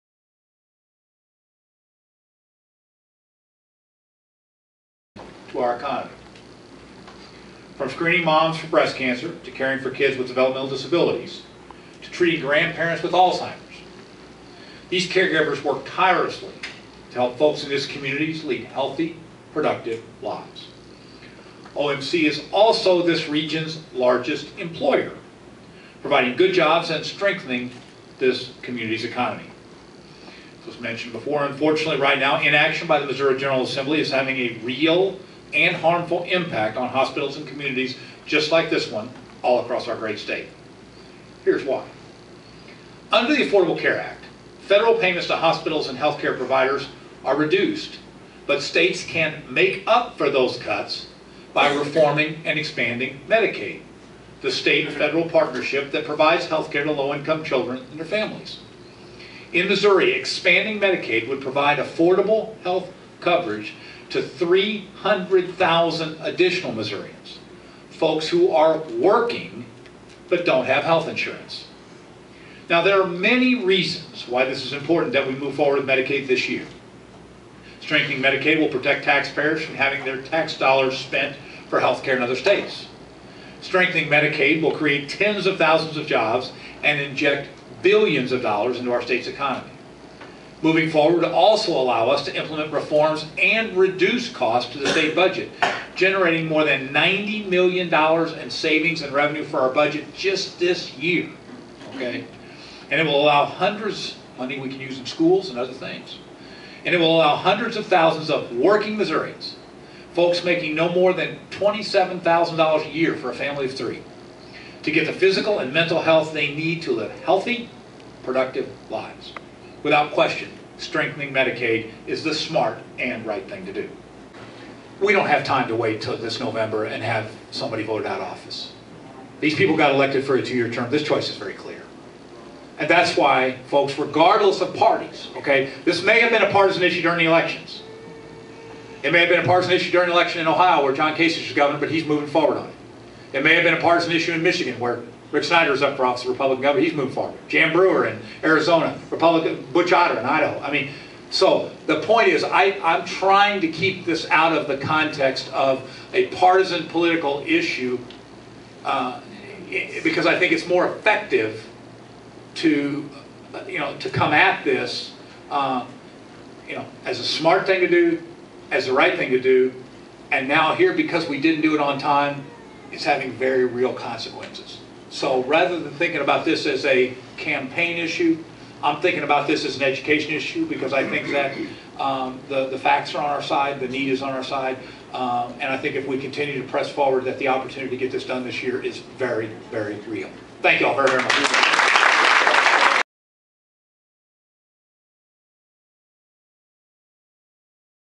Ozark Radio News - Gov. Jay Nixon Speaking at OMC Parkway Center - 4/2/14
Missouri Gov. Jay Nixon discussed the need for Medicaid expansion at the OMC Parkway Center on Wednesday, March 2.